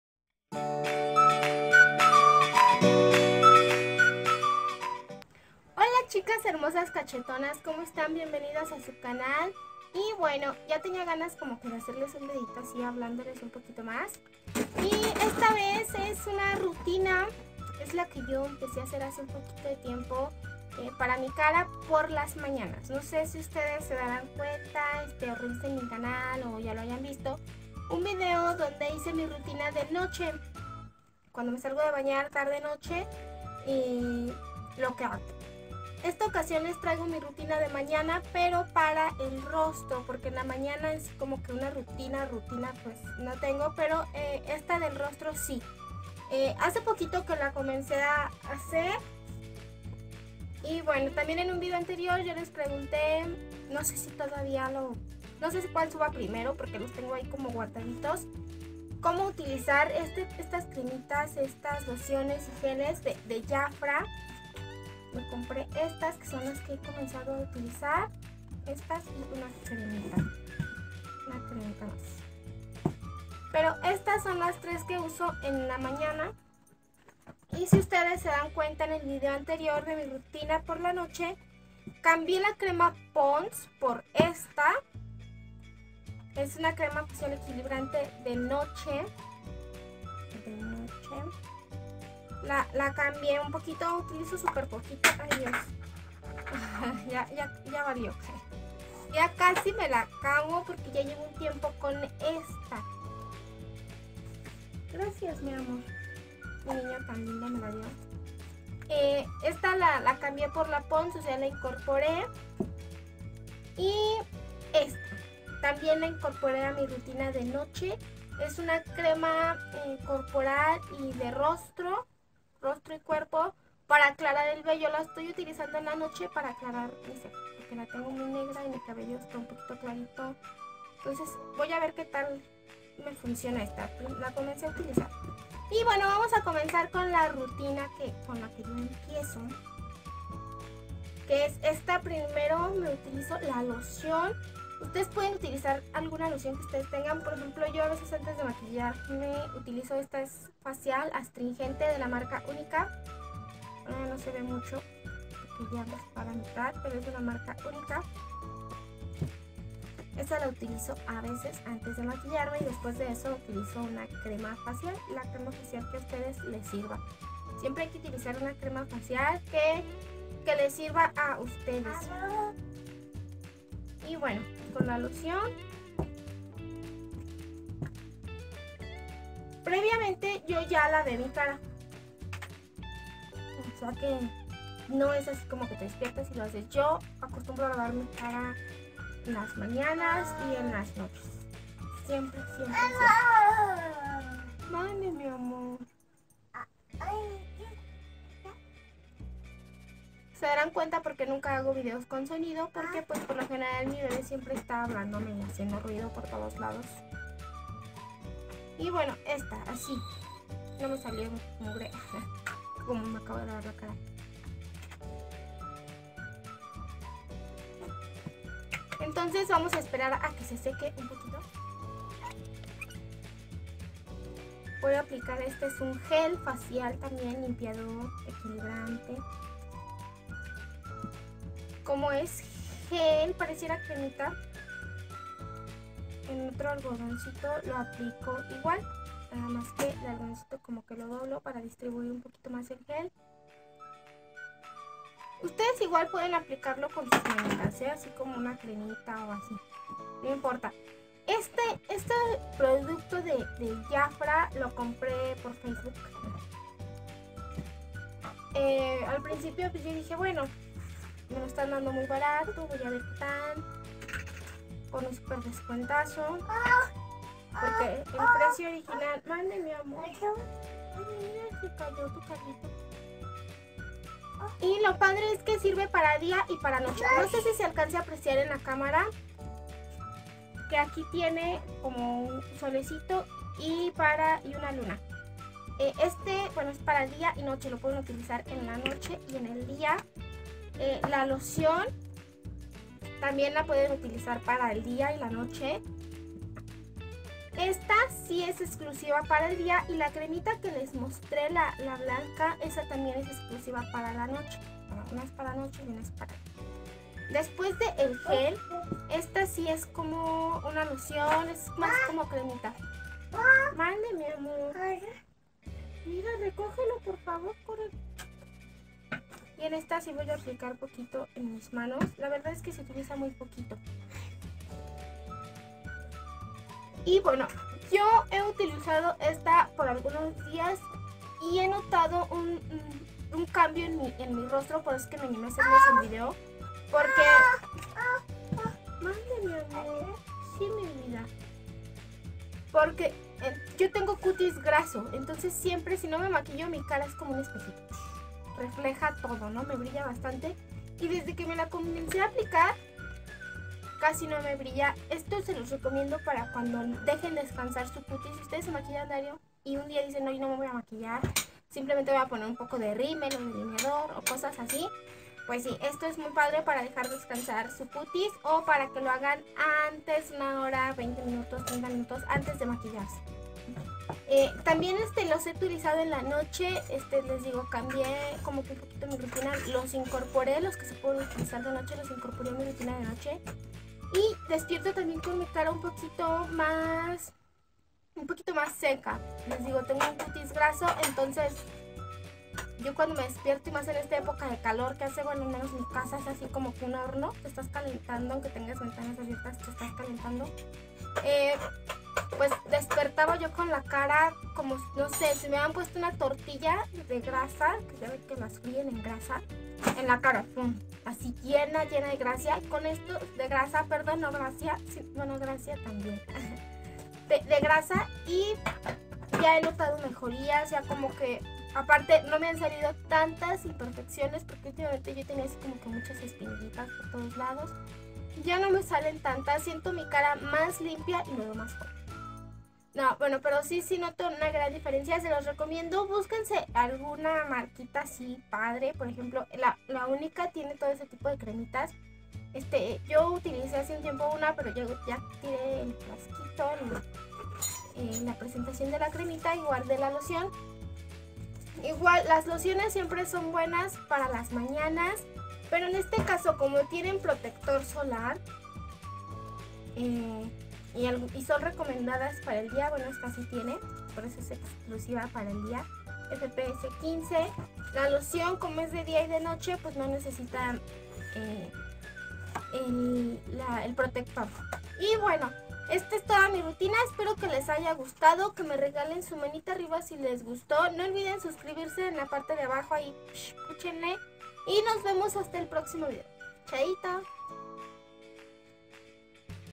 Mi rutina de ROSTRO POR LA MAÑANA 👧 || DanhyVlog😺
Chicas les comparto lo que hago en las mañanas y la verdad me ha dejado muy bonitos resultados en mi cara.